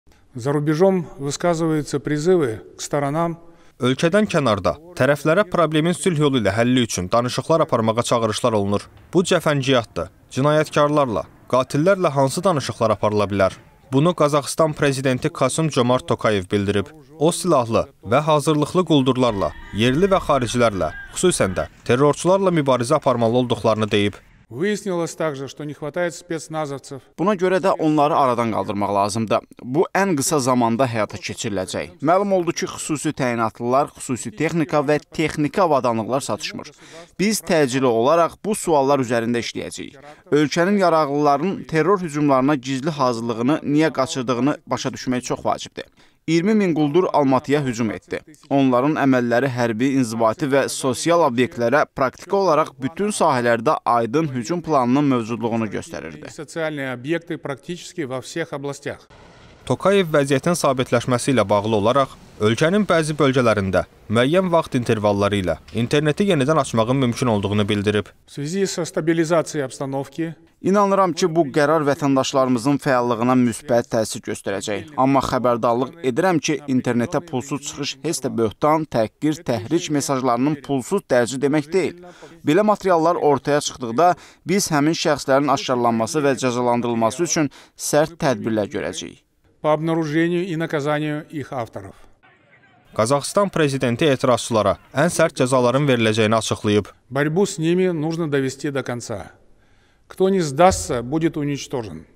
Qazaxıstan prezidenti etirazçılara ən sərt cəzaların veriləcəyini açıqlayıb - Baku TV
Ölkədən kənarda tərəflərə problemin sülh yolu ilə həlli üçün danışıqlar aparmağa çağırışlar olunur. Bu, cəfəngiyyatdır. Cinayətkarlarla, qatillərlə hansı danışıqlar aparıla bilər?!
Bunu Qazaxıstan Prezidenti Kasım-Jomart Tokayev bildirib. O, silahlı və hazırlıqlı quldurlarla, yerli və xaricilərlə, xüsusən də terrorçularla mübarizə aparmalı olduqlarını deyib.
Tokayev vəziyyətin sabitləşməsi ilə bağlı olaraq ölkənin bəzi bölgələrində müəyyən vaxt intervalları ilə interneti yenidən açmağın mümkün olduğunu bildirib.
Qazaxıstan Prezidenti etirazçılara ən sərt cəzaların veriləcəyini açıqlayıb.

Bizi Sosial şəbəkələrdə izləyə bilərsiniz.